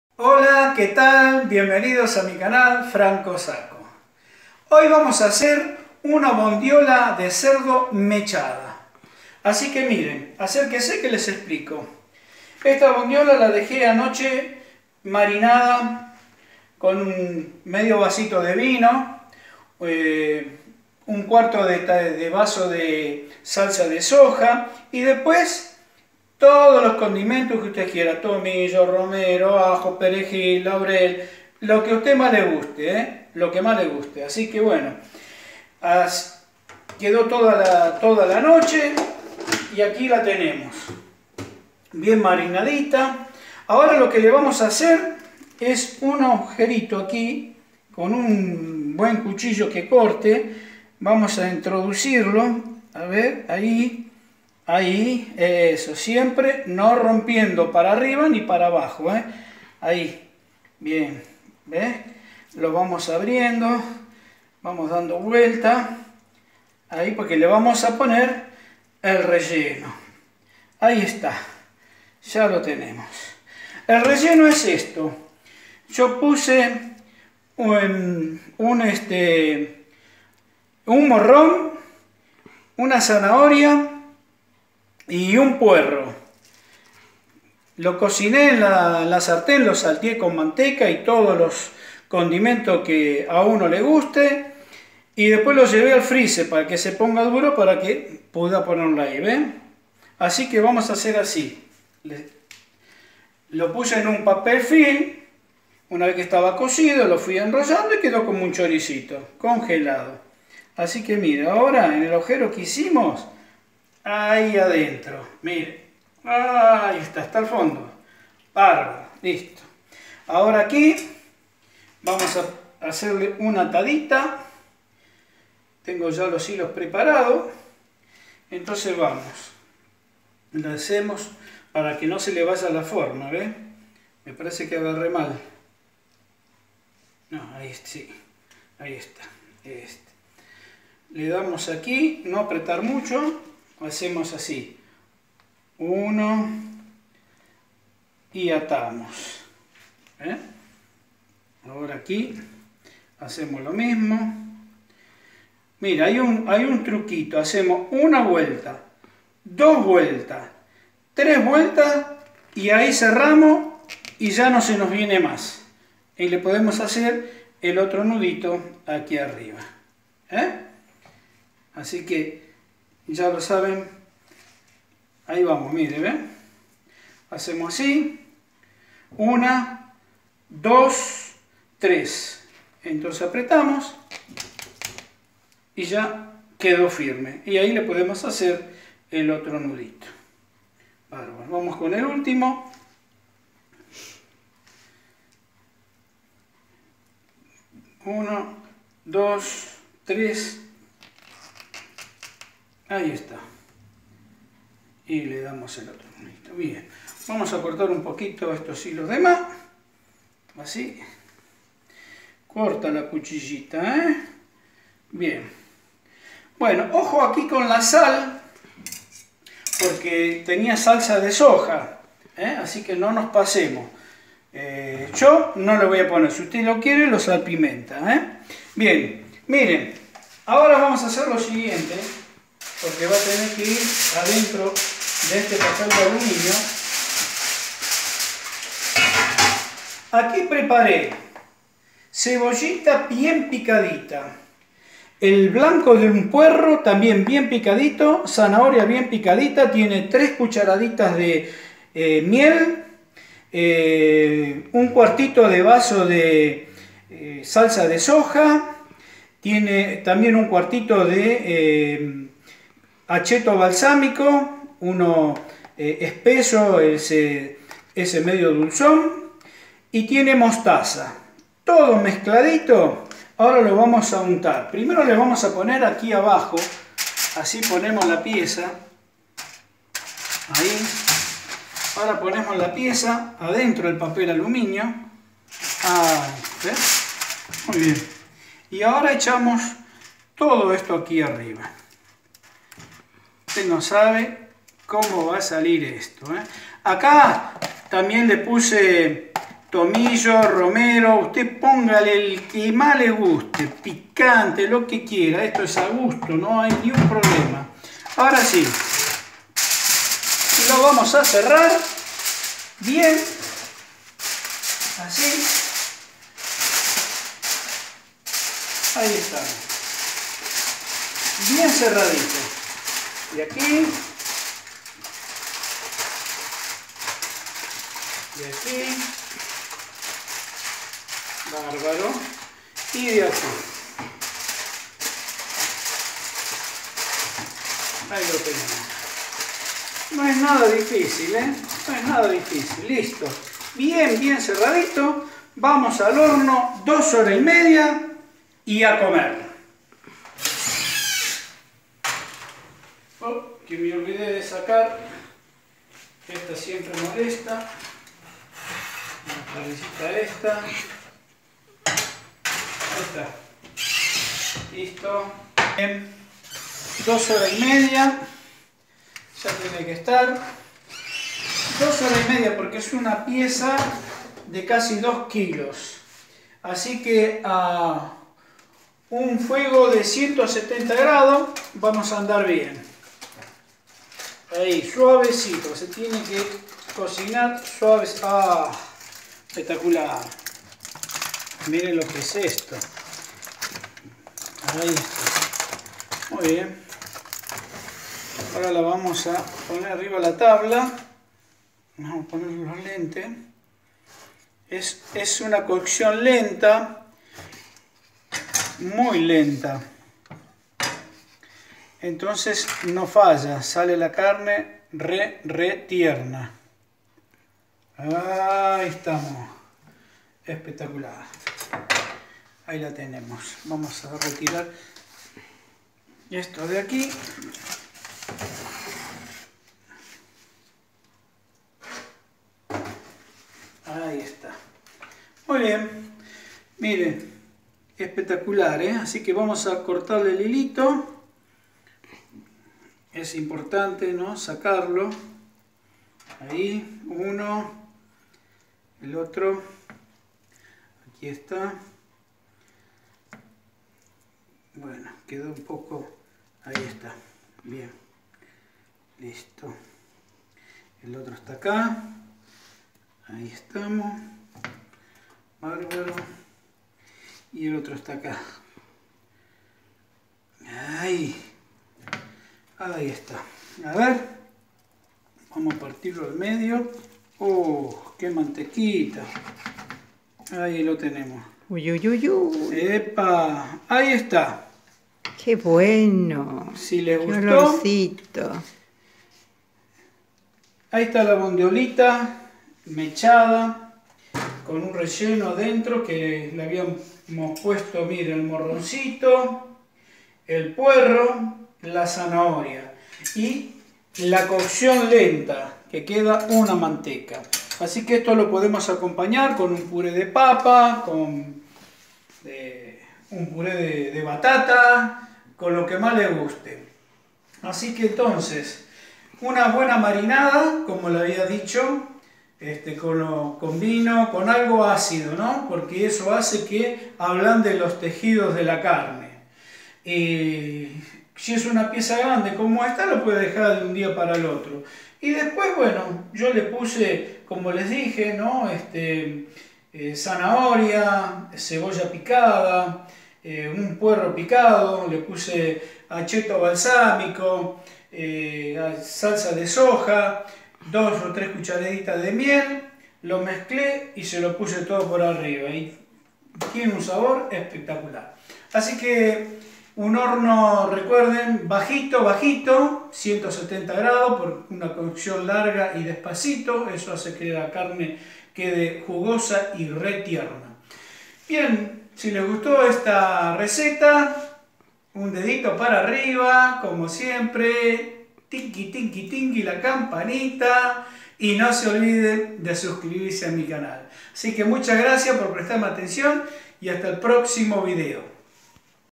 Bondiola braceada rellena
La bondiola la deje macerada toda la noche con medio vaso de vino tinto, 1/4 vaso de salsa de soja, 1/4 vaso de aceto balsámico tipo reducción, 2 cucharadas de miel y distintas especies. Luego se mecha con morrón, zanahoria y puerro que previamente sofrei con manteca, aceite y especias. En un papel film arme como un chorizo con esos vegetales y lo congelé en el freezer hasta que quedó duro. Ese chorizo de vegetales se introduce en la carne a la cual le practiqué un orificio en el medio. Envolví la carne en papel aluminio, al cual previamente unte con una salsa,  con una cucharada de miel, dos dientes de ajo picado,  cebolla picada finita, el blanco de un puerro bien picadito, media zanahoria bien picadita, una cucharada de aceto balsámico reducción, una cucharada de salsa de soja y una cucharada de mostaza y especias a gusto. Luego al horno con el papel aluminio bien cerradito dos horas y medio a 170 grados. Y a disfrutar